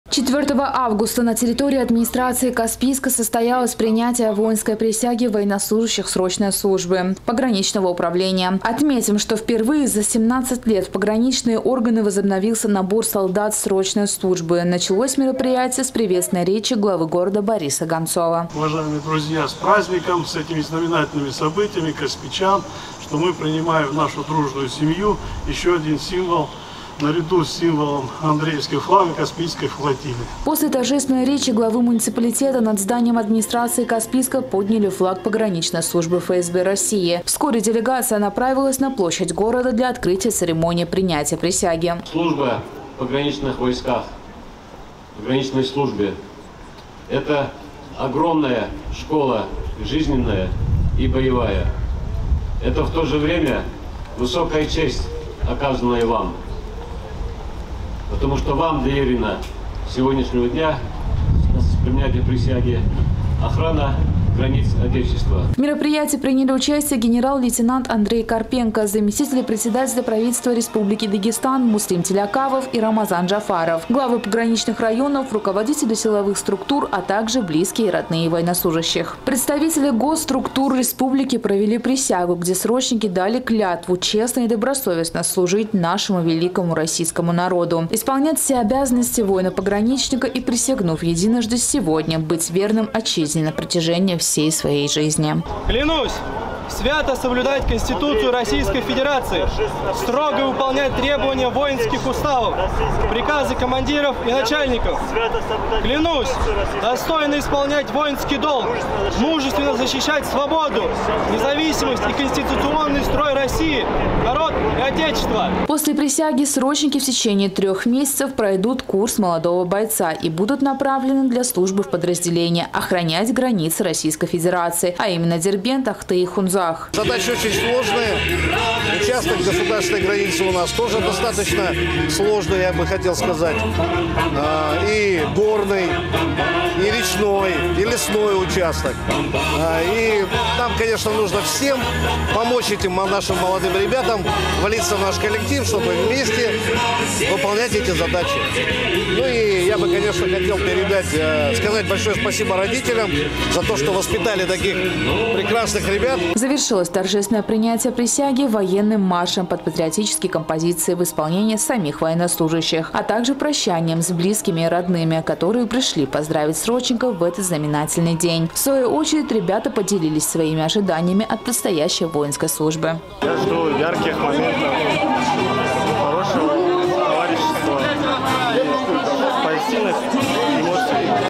Принятие присяги военнослужащих срочной службы Пограничного управления прошло в Каспийске
4 августа на территории администрации Каспийска состоялось принятие воинской присяги военнослужащих срочной службы Пограничного управления. Отметим, что за впервые за 17 лет в Пограничные органы возобновился набор солдат срочной службы. Началось мероприятие с приветственной речи главы города Бориса Гонцова.

После торжественной речи главы муниципалитета над зданием администрации Каспийска подняли флаг пограничной службы ФСБ России. Вскоре делегация направилась на площадь города для открытия церемонии принятия присяги.